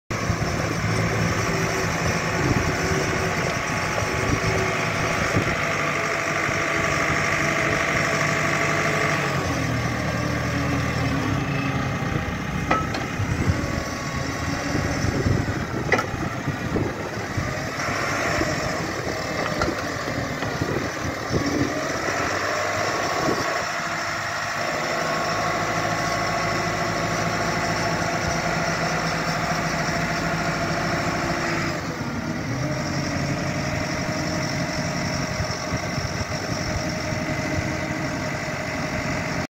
2013 JCB 534/140 Teleporter For Auction
2013 JCB 535/140 Telescopic Handler, Bucket, Sway, 5820 Hours For Auction 3rd March 2021. for more information please